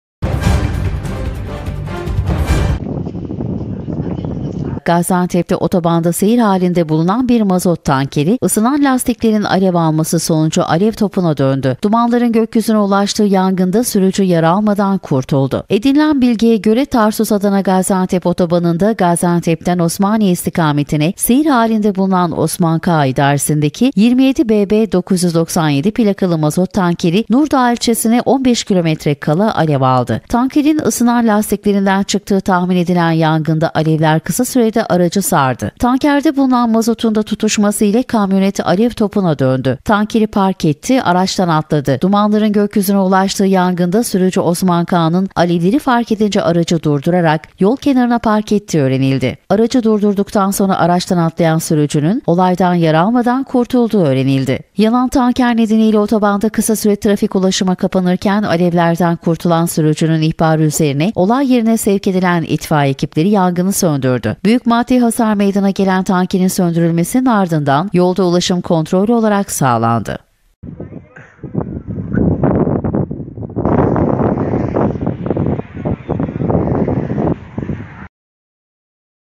ALEV TOPUNA DÖNEN MAZOT TANKERİ KAMERADA
Gaziantep'te otobanda seyir halinde bulunan bir mazot tankeri, ısınan lastiklerin alev alması sonucu alev topuna döndü. Dumanların gökyüzüne ulaştığı yangında sürücü yara almadan kurtuldu
Edinilen bilgiye göre, Tarsus Adana Gaziantep (TAG) otobanında Gaziantep'ten Osmaniye istikametine seyir halinde bulunan Osman K. idaresinde 27 BB 997 plakalı mazot tankeri, Nurdağı ilçesine 15 kilometre kala alev aldı. Tankerin ısınan lastiklerinden çıktığı tahmin edilen yangında alevler kısa sürede aracı sardı. Tankerde bulunan mazotun da tutuşması ile kamyonet alev topuna döndü.
Tankeri park etti, araçtan atladı
Dumanların gökyüzüne ulaştığı yangında sürücü Osman K.'nin alevleri fark edince aracı durdurarak, yol kenarına park ettiği öğrenildi. Aracı durduktan sonra araçtan atlayan sürücünün, olaydan yara almadan kurtulduğu öğrenildi.
Yanan tanker nedeniyle otobanda kısa süre trafik ulaşıma kapanırken, alevlerden kurtulan sürücünün ihbarı üzerine olay yerine sevk edilen itfaiye ekipleri yangını söndürdü.
Büyük maddi hasar meydana gelen tankerin söndürülmesinin ardından yolda ulaşım kontrollü olarak sağlandı.